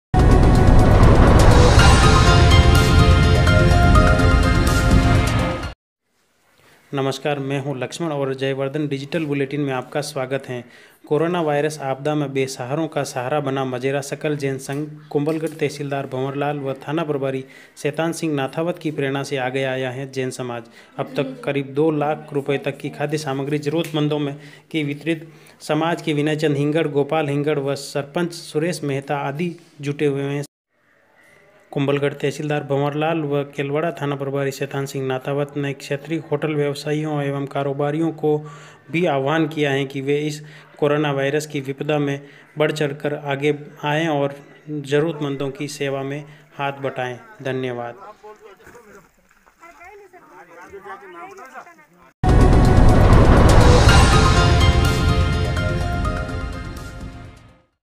Corona virus influence ! Society become a help to the needy
कुंभलगढ़ @ कोरोना वायरस संक्रमण के चलते 14 अप्रैल तक देश में लॉकडाउन की वजह से बेरोजगार होने से घर गुजारे पर संकट खड़ा होने पर जरूरतमंद परिवारों की मदद के लिए मजेरा सकल जैन समाज आगे आया है। कुंभलगढ़ तहसीलदार भंवरलाल व केलवाड़ा थाना प्रभारी शैतानसिंह नाथावत की पे्ररणा से समाज के विनयचंद हिंगड़, गोपाल हिंगड़ व सरपंच सुरेश मेहता सेवा में जुटे हुए हैं। केलवाड़ा कस्बे के साथ ही आस पास के गांवों में जरूरतमंद लोगों को चिह्नित कर नियमित उन्हें खाद्य सामग्री उपलब्ध कराई जा रही है। अब तक करीब दो लाख रुपए तक की खाद्य सामग्री वितरित की जा चुकी है।

Covid-19, Corona Mewadi Songs, Rajasthan Corona virus hindi news,